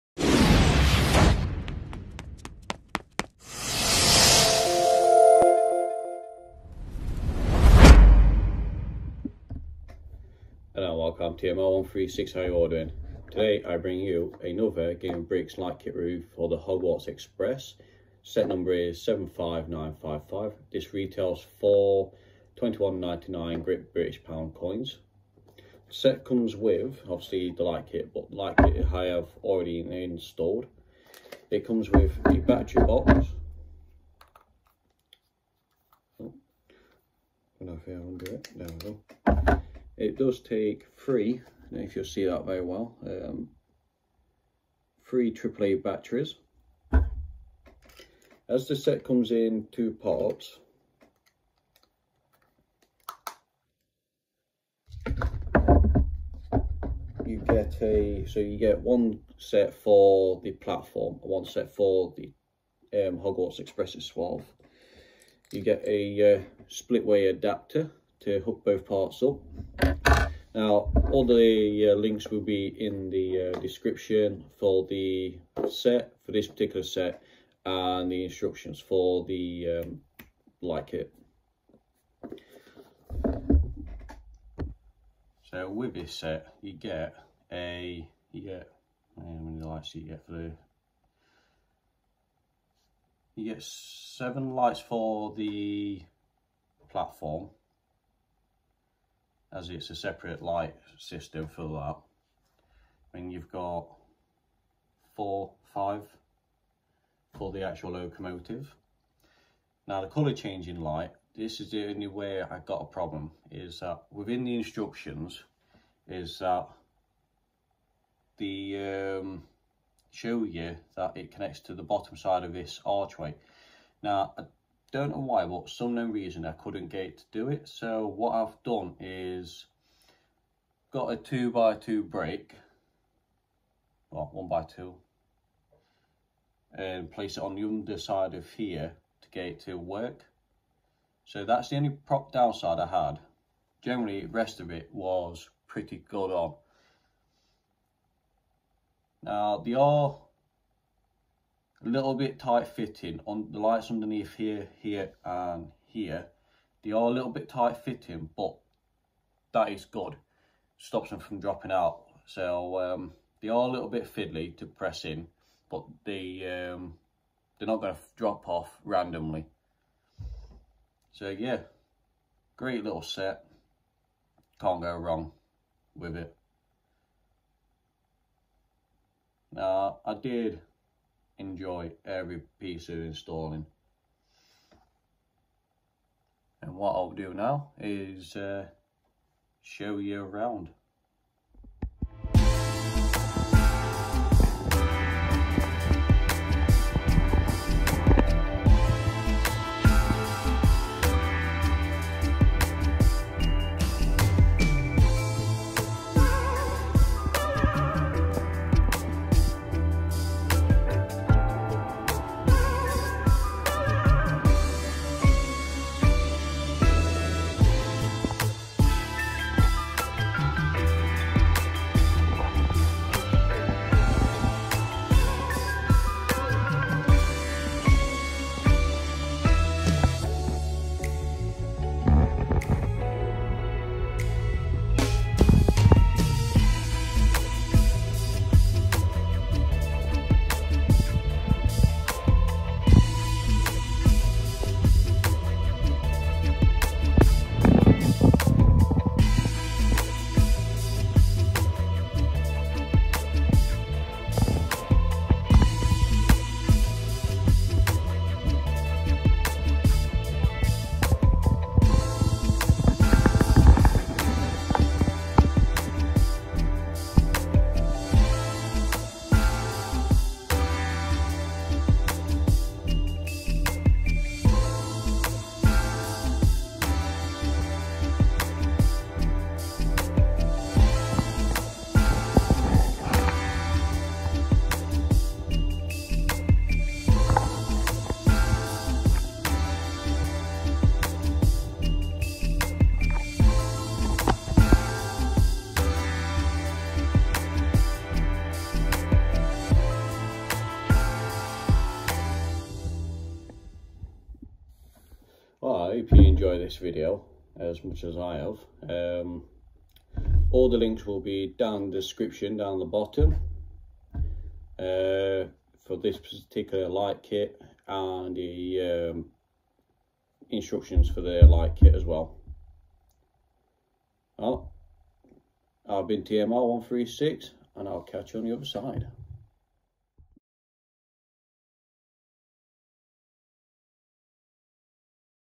Lego hogwarts express light kit review set 75955
Hogwarts express light kit comes with a battery pack will need 3 Triple￼ aaa batteries there is a two way Adapter￼ for connecting both platform and hogwarts express as the set is in 2 part’s the price for this set is £21.99 here in the uk 🇬🇧

:instructions